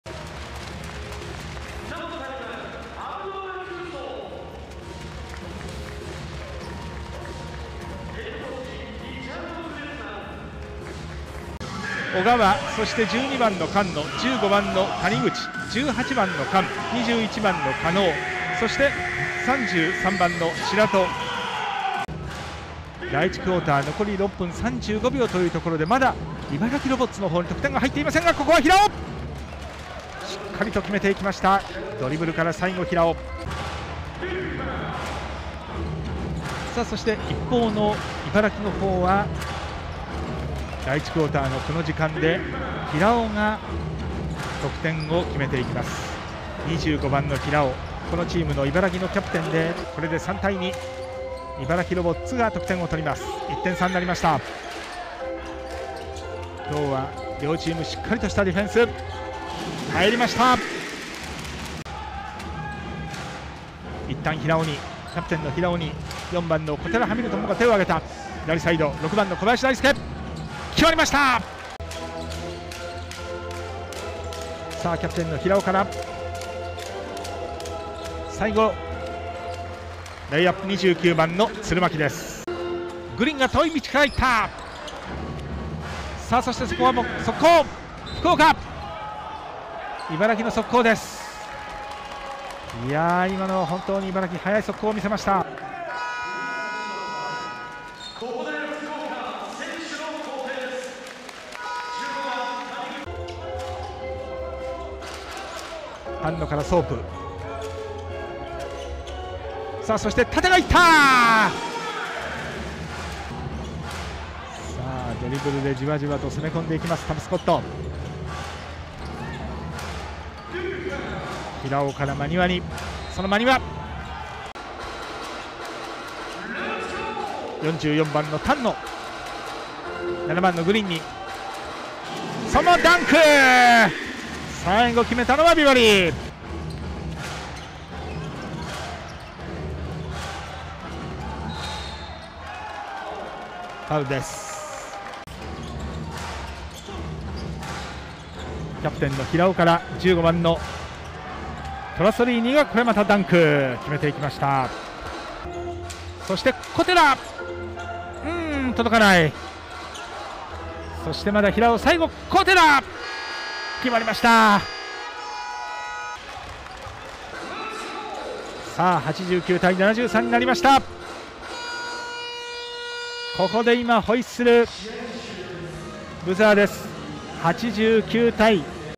【ハイライト】ライジングゼファー福岡vs茨城ロボッツ｜B2第21節GAME1｜02.13.2021 プロバスケ (Bリーグ)
Watch the Game Highlights from Rizing Zephyr Fukuoka vs. Ibaraki Robots, 02/13/2021